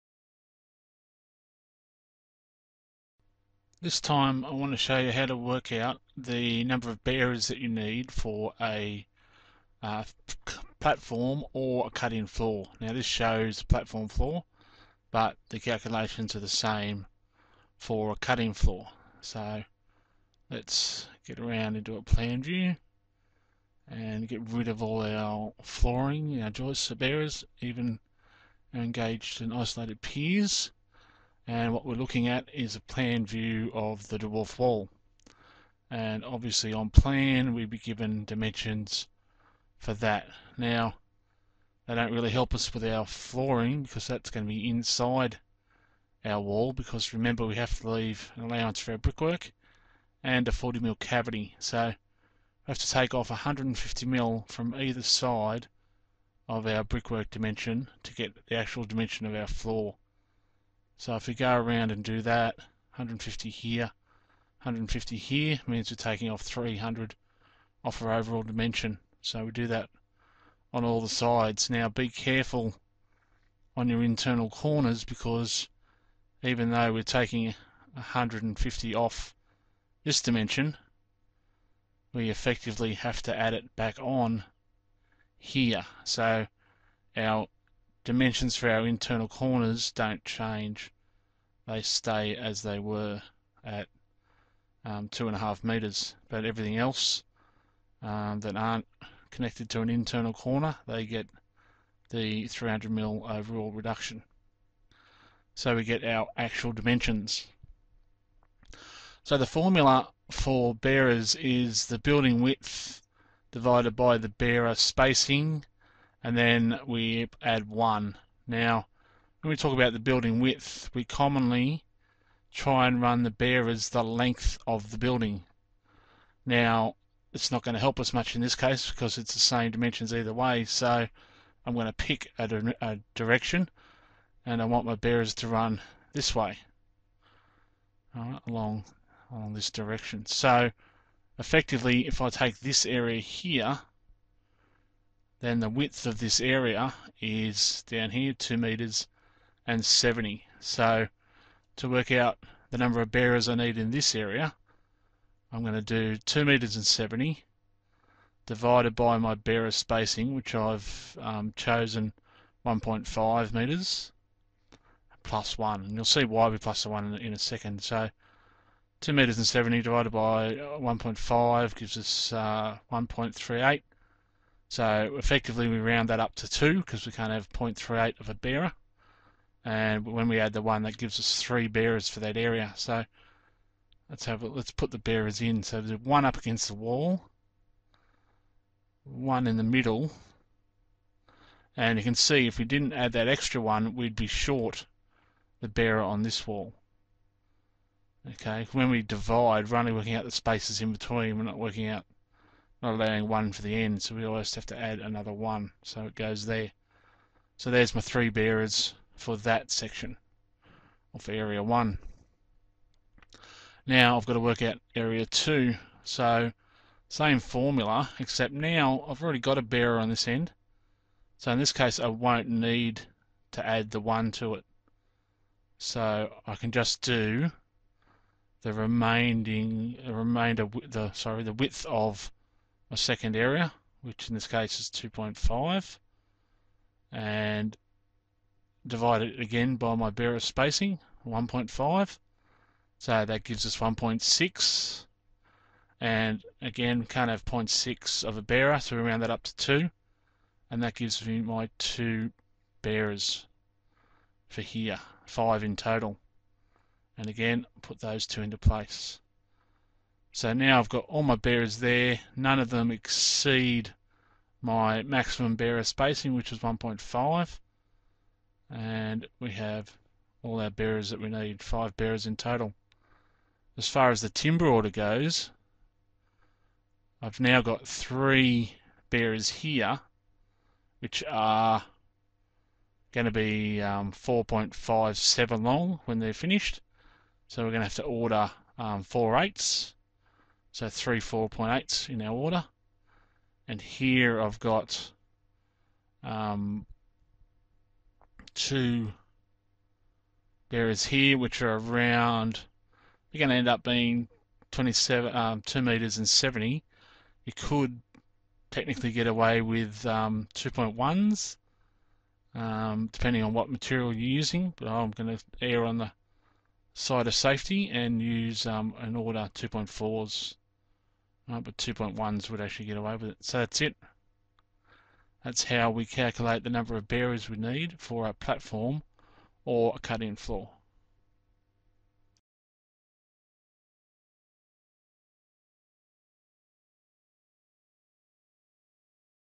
Calculating Bearers
This video shows you how to calculate the number of Bearers required to construct a Bearers and Joist floor.